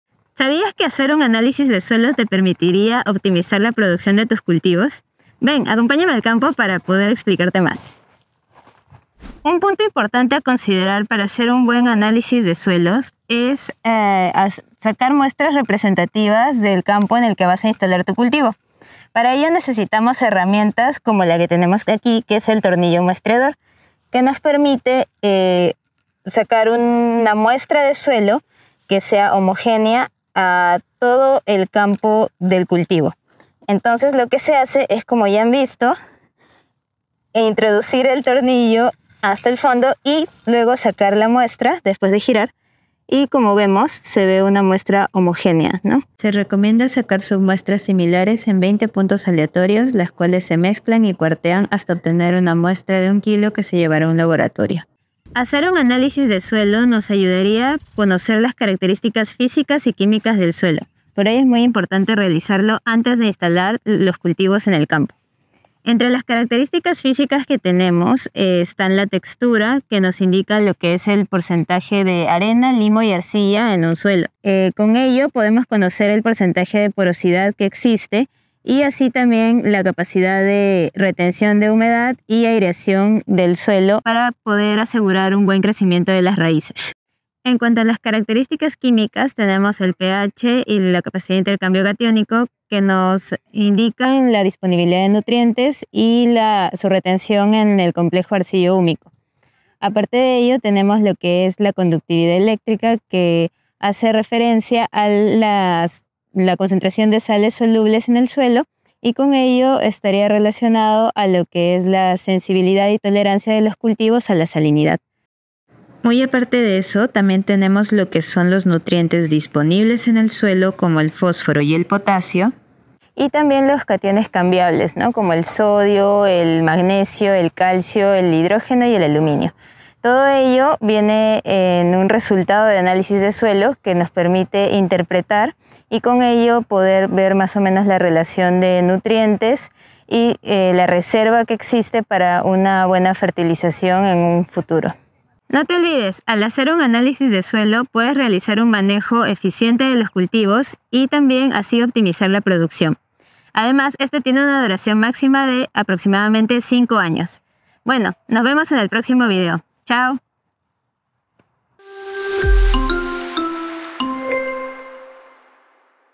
Importancia del análisis de suelo
Análisis de Suelo
¿Sabes su importancia?
El área técnica de Molinos & Cía. nos explica la importancia de hacer análisis de suelo, para plantear un manejo agronómico adecuado, eficiente y sostenible antes de instalar un cultivo.
hashtag#creciendojuntos hashtag#fertilizantes hashtag#peru hashtag#suelo